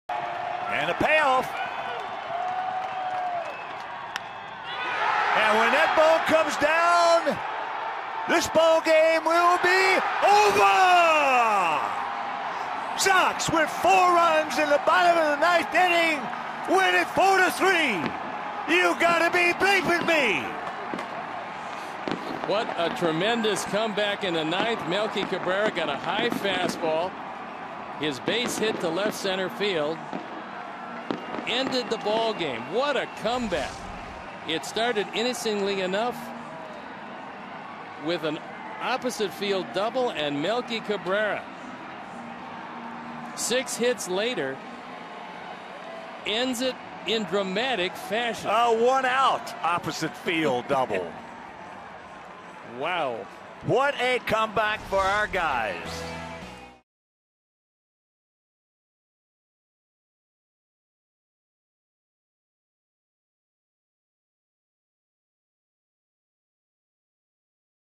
Melky delivers a walk-off single in 9th
4/20/15: Melky Cabrera drills a walk-off single to left-center field in the 9th, giving the White Sox a 4-3 win over the Indians